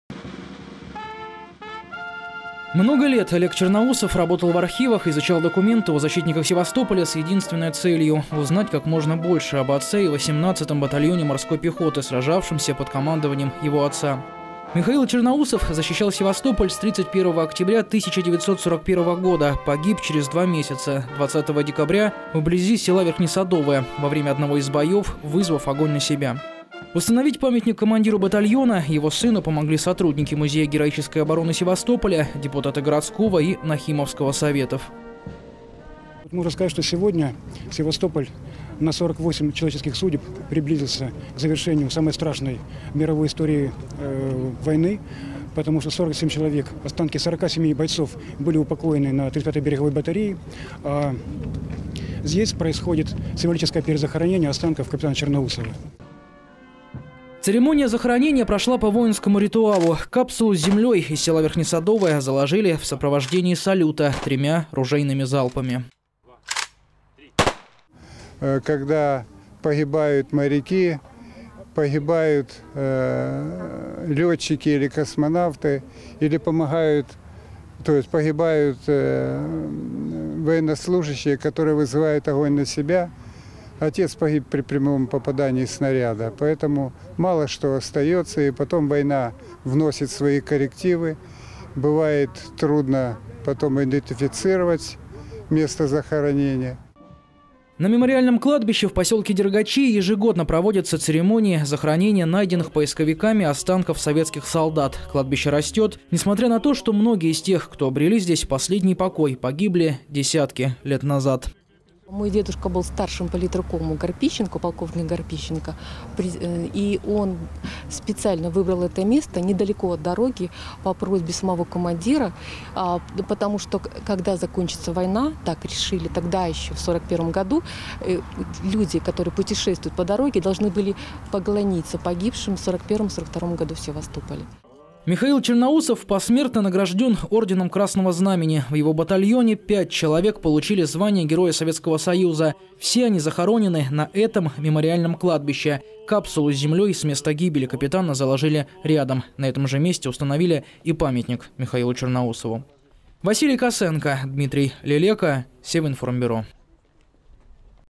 30 10 2014 СИМВОЛИЧЕСКОЕ ПЕРЕЗАХОРОНЕНИЕ КАПИТАНА ЧЕРНОУСОВА
В поселке Дергачи на мемориальном кладбище советских воинов в четверг прошло символическое перезахоронение участника второй обороны Севастополя, капитана Михаила Черноусова. Символическое не только потому, что связано с важной страницей истории Севастополя  и имеет отношение к каждому, кто чтит память советских воинов, но и

потому,  что перезахоронили  не останки офицера, а землю с места гибели капитана Черноусова. Церемонию провели по воинскому ритуалу.